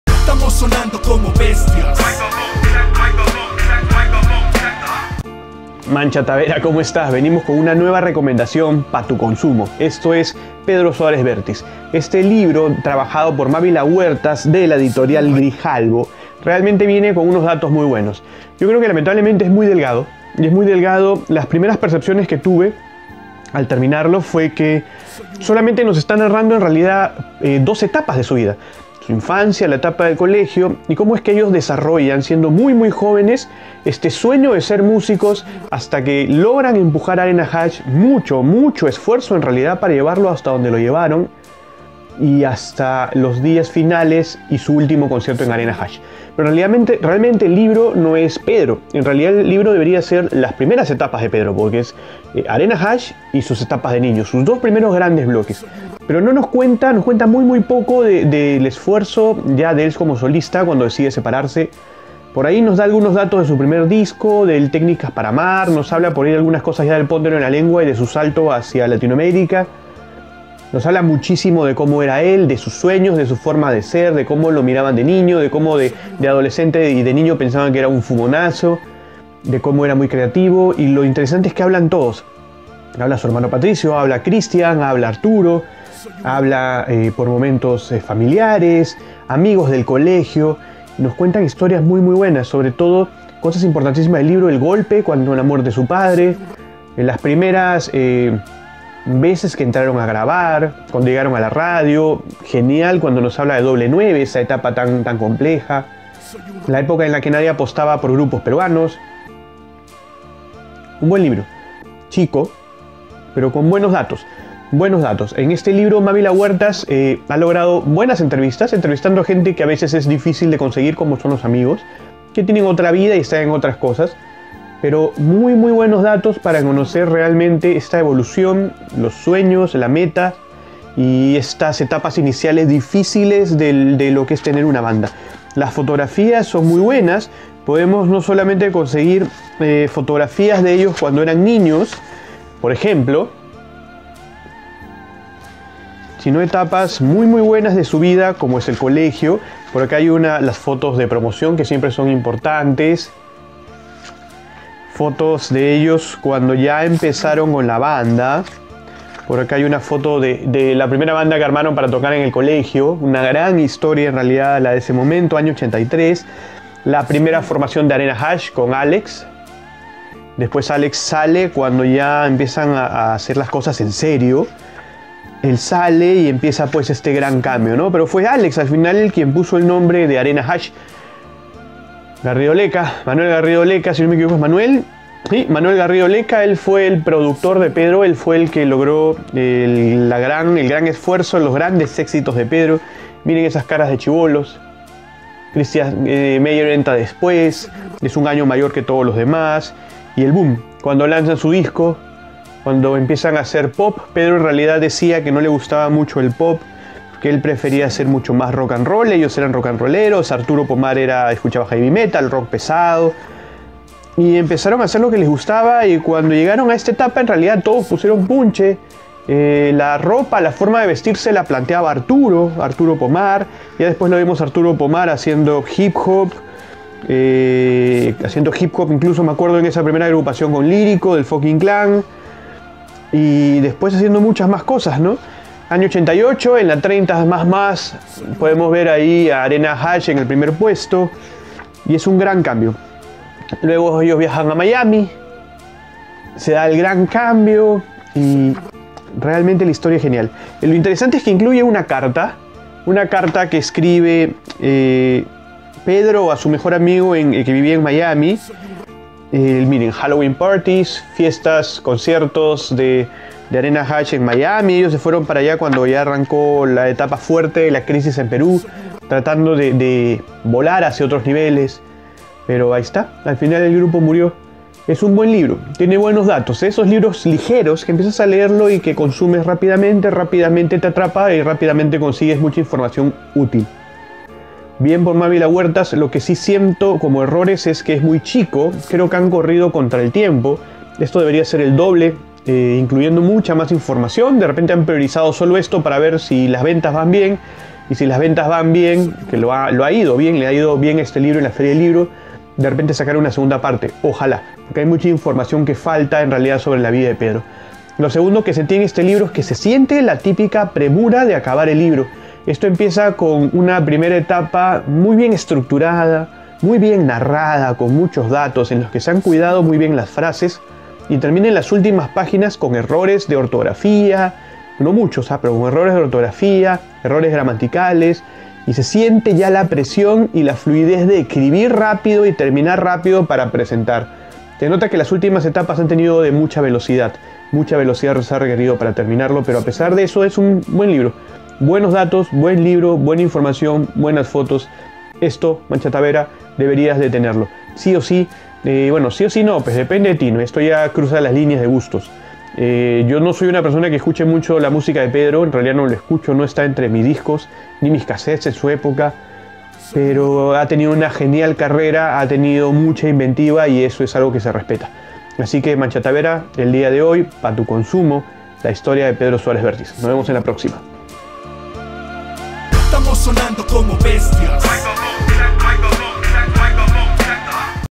Pedro Suárez Vértiz, el libro de Mávila Huertas - Pa tu consumo 005
Esta semana te recomendamos pa tu consumo el libro que escribió Mávila Huertas sobre Pedro Suárez Vértiz. En el encontraras muchos datos interesantes sobre sus primeras dos etapas de la vida: Niñez y Arena Hash.